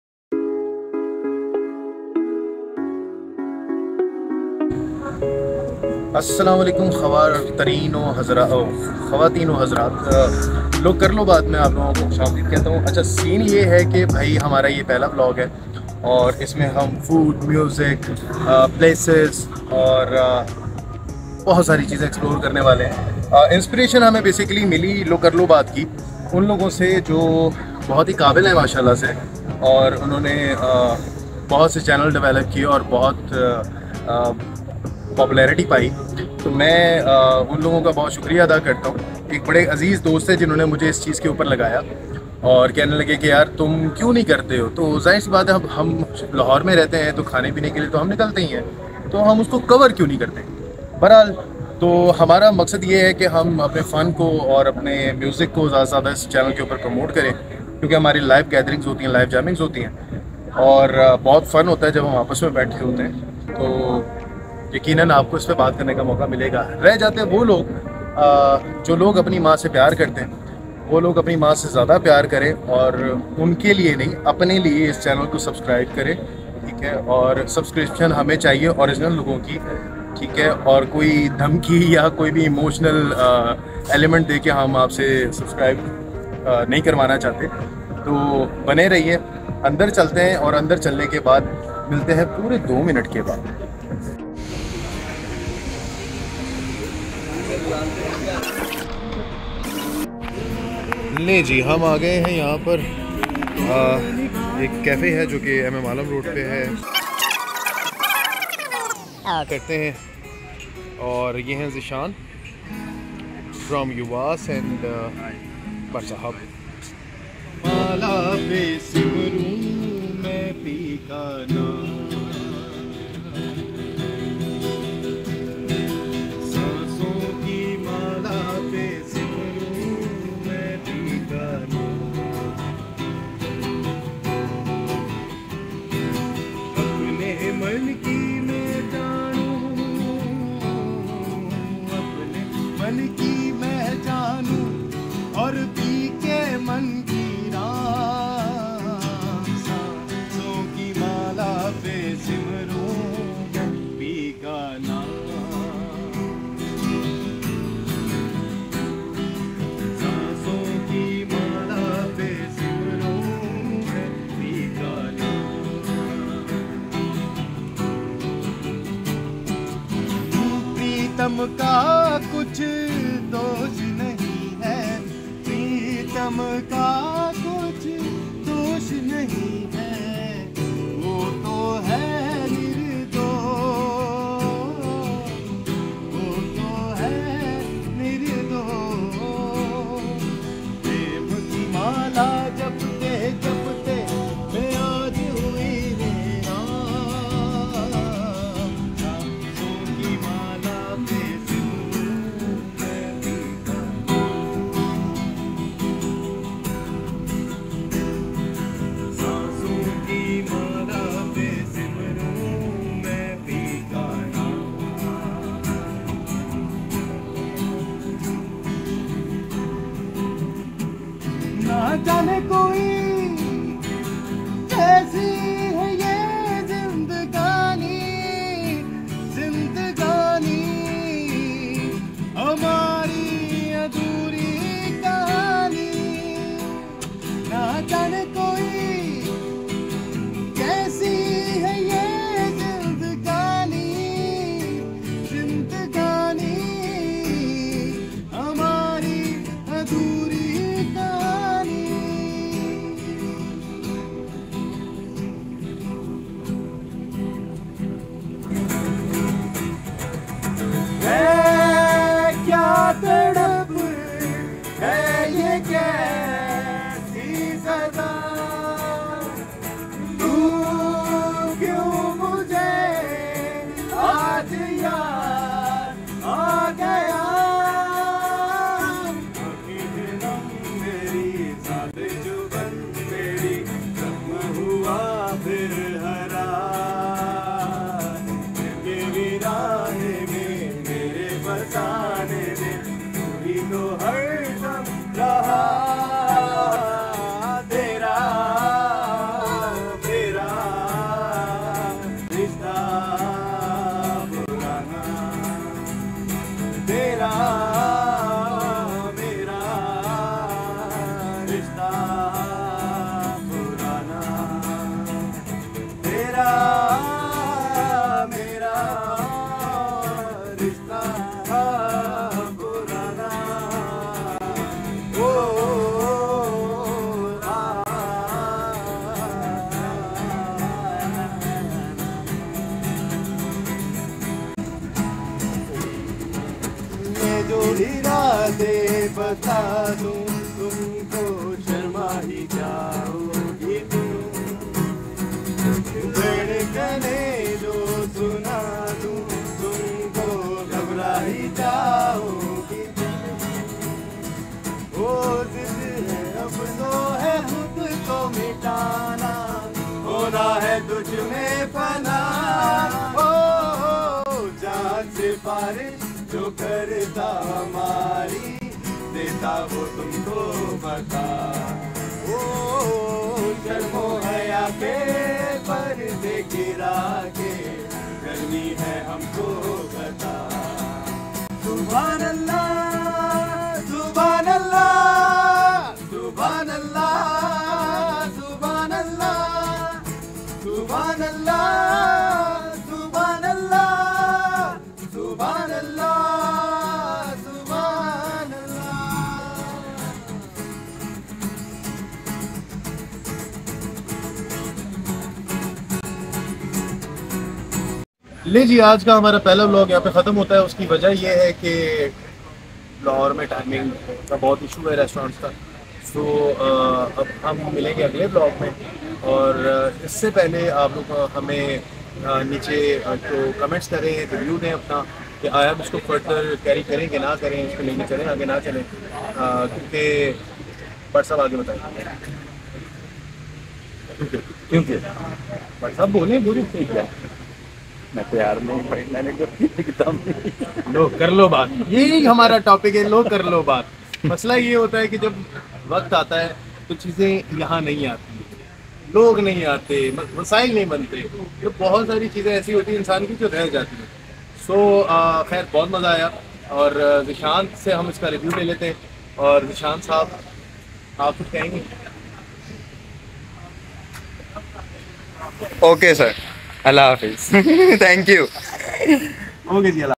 لو کر لو بات 🤫 🙈💯 | LO KARLO BAAT WITH ZAMEER KHAN
This Is My First Vlog (Intro)   لو کر لو بات
is About Friends Gathering with Music Jam Interviews Food Fun Channel..
Location Lahore MM Alam Road Near Hussain Chowk Roundabout .

You Can Catch Us.